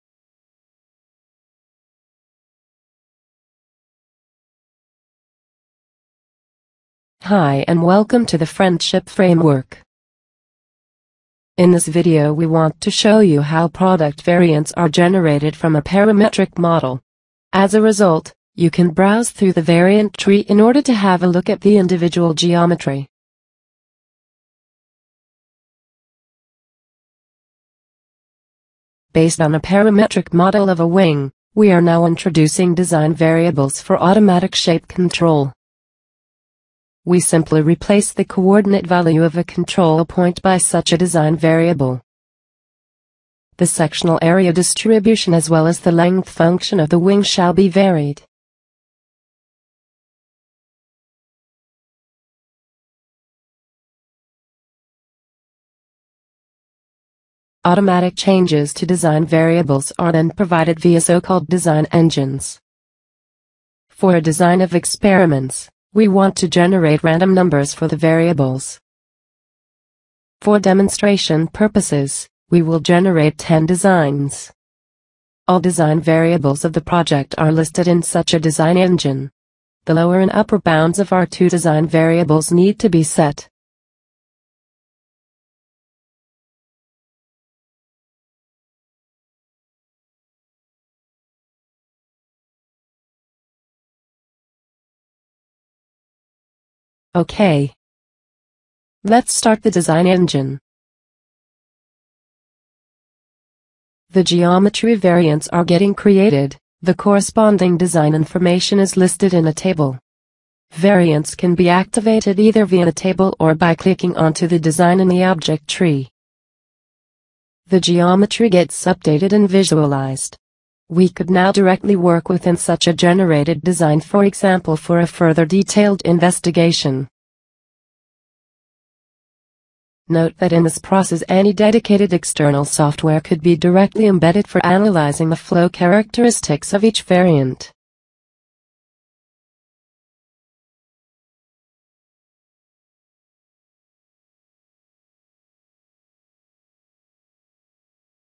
Automated variation of geometry for CFD-based optimizations
In this short video tutorial product variants are created from a parametric model. For demonstration purposes, a basic wing design is utilized that has been created before in CAESES (formerly called FRIENDSHIP-Framework), too. This technique is specialized for automated processes so that this kind of models can be directly coupled to commercial or non-commercial CFD-packages for feasible and efficient shape optimizations.